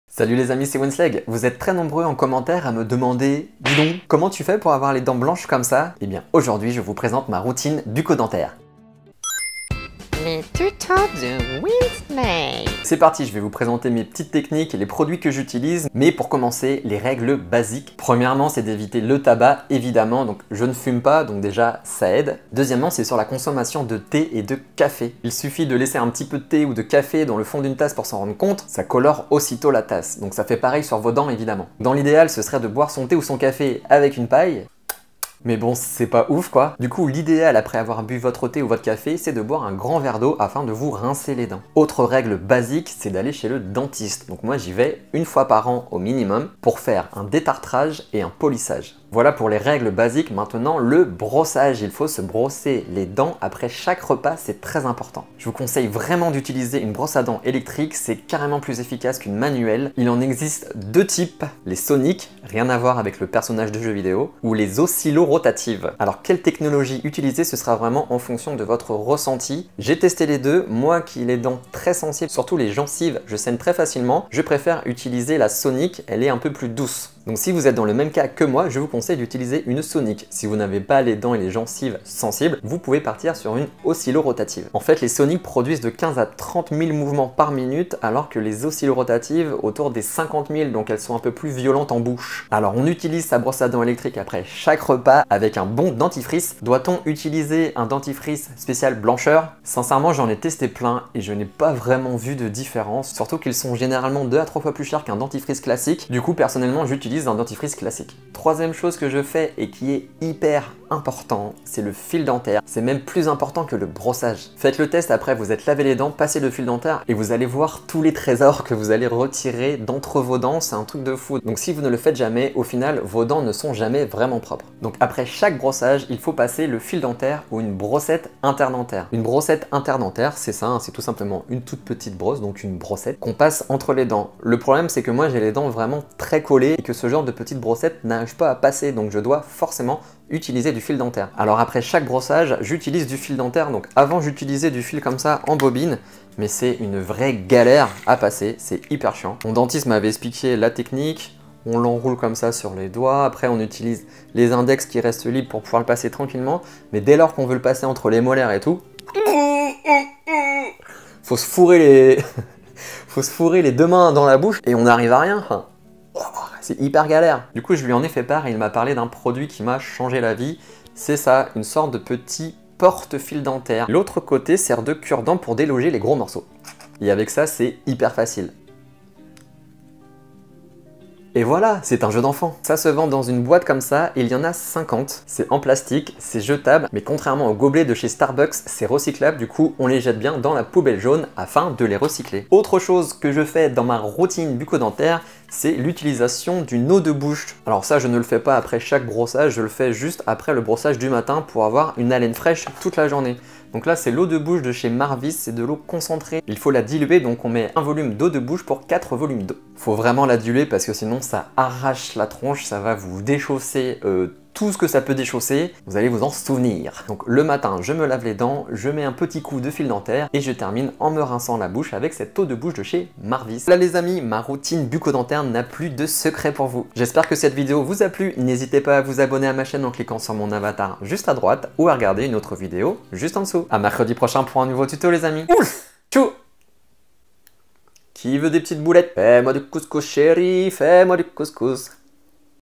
COMMENT AVOIR LES DENTS BLANCHES : ma routine bucco-dentaire !
Découvrez toutes mes astuces pour avoir les dents blanches facilement !

• Certains produits présentés peuvent être des produits reçus gratuitement, cela ne change en rien mes avis sur ces derniers.